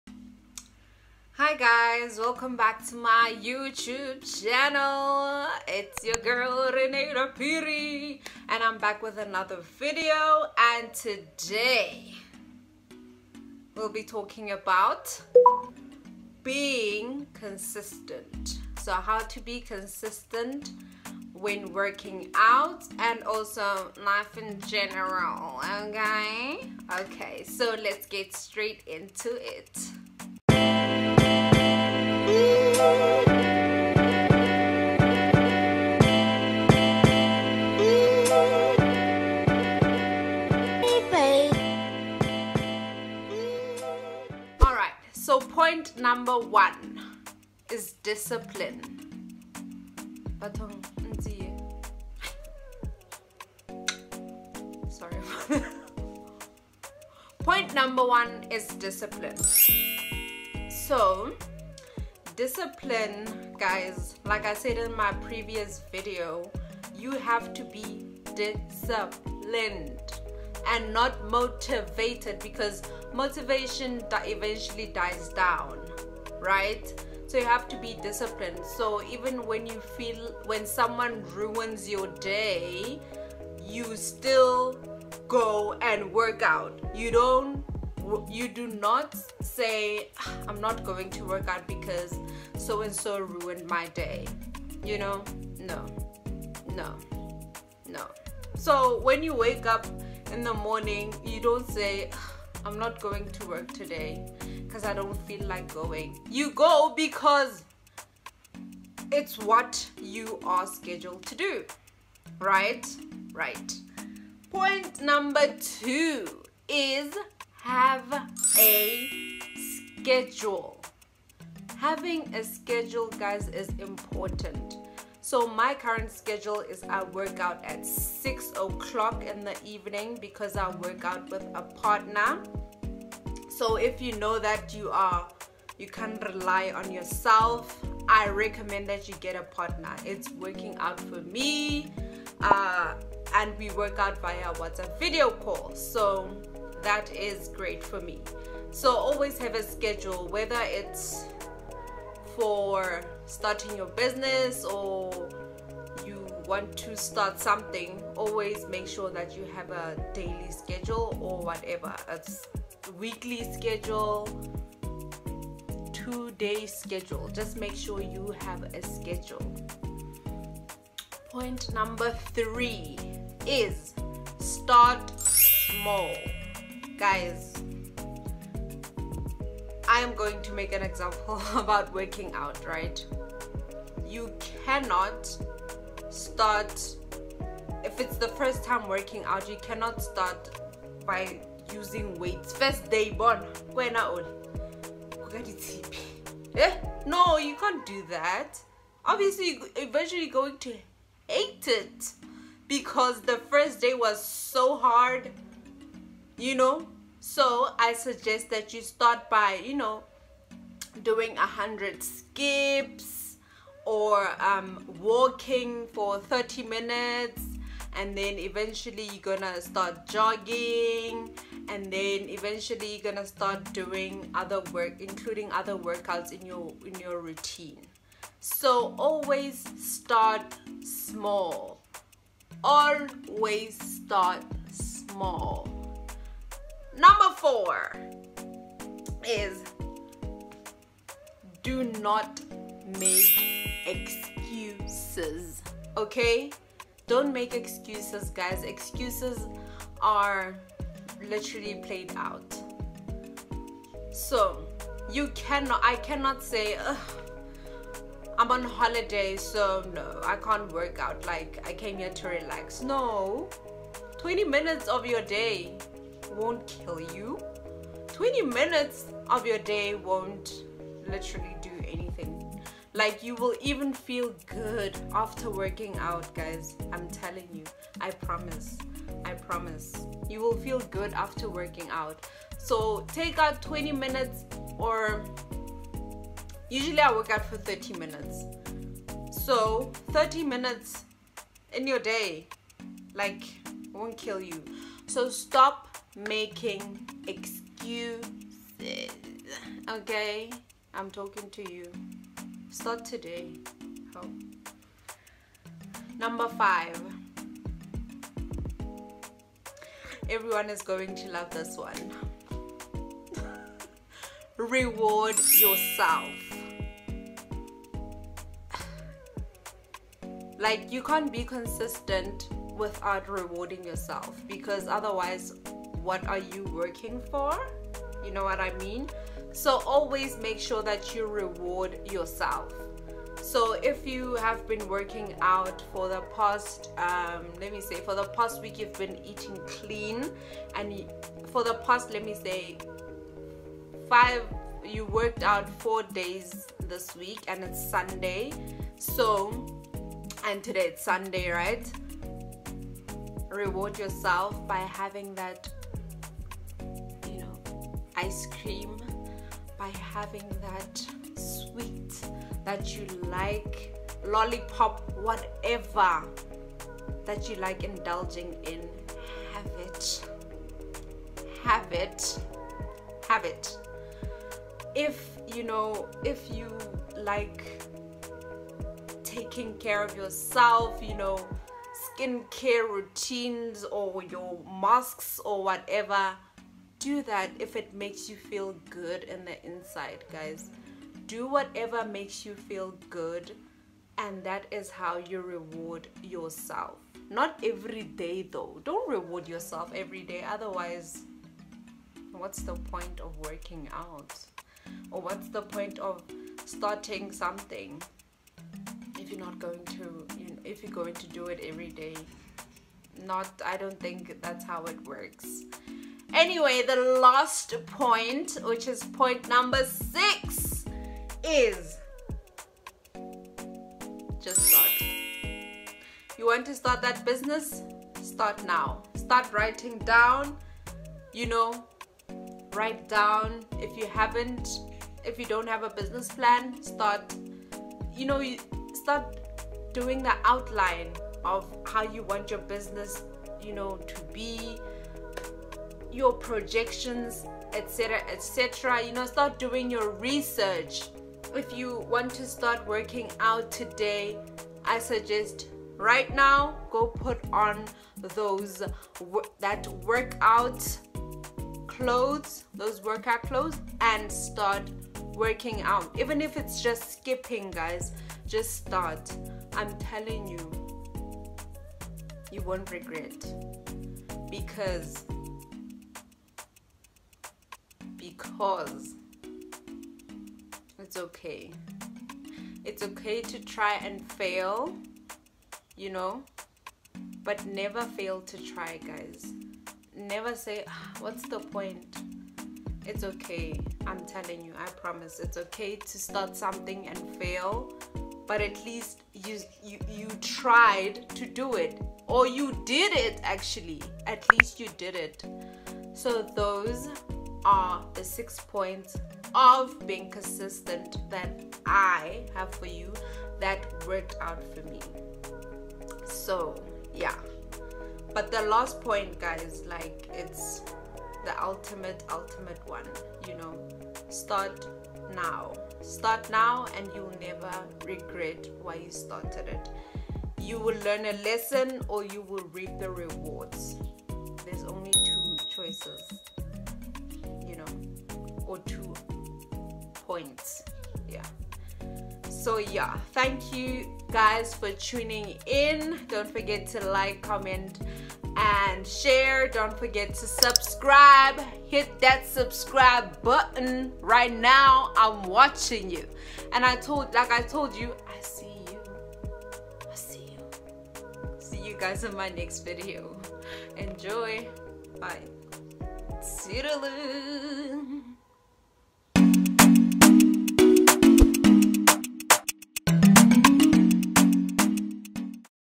CONSISTENCY: IN 6 EASY STEPS, GYM, WORKOUT & LIFE IN GENERAL| SA YOUTUBER, RENEILWE PHIRI
Hey guys

Today we talk about consistency, when gyming & life In general. Enjoy!